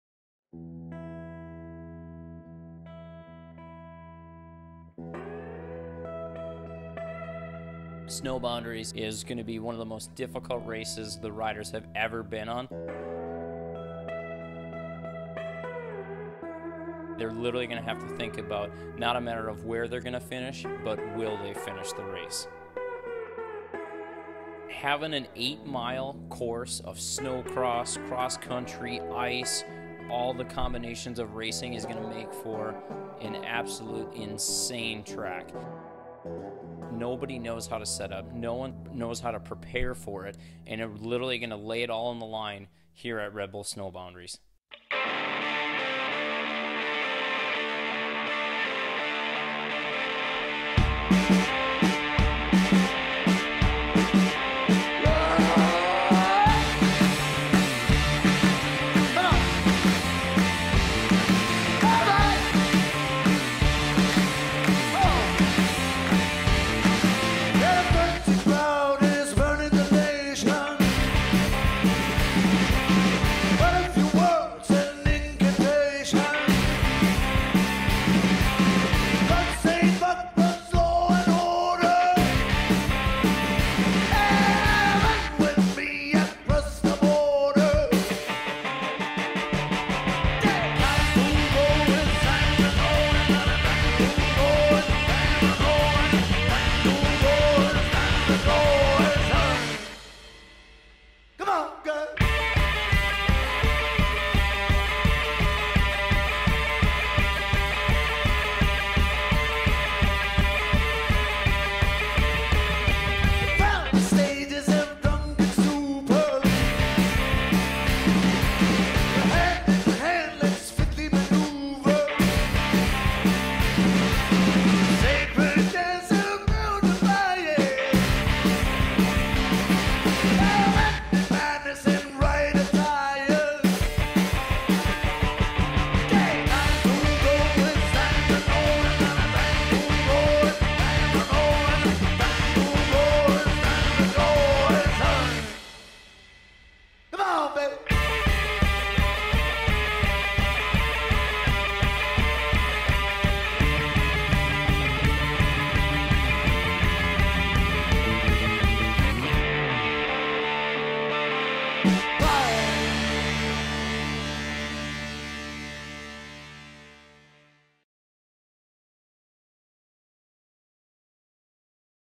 The Ultimate Snowmobile Race | Red Bull Snow Boundaries 2016
There’s snocross, and then there’s Red Bull Snow Boundaries. Hillclimbs, ice sprints, massive jumps and cross-country all eagerly wait to swallow up competitors throughout the eight-mile course. It’s a hard enduro on the snow, a race that tests mind, body and machine to the point of breakdown. Click play and take a look back at the wild snowmobile action from Elk River!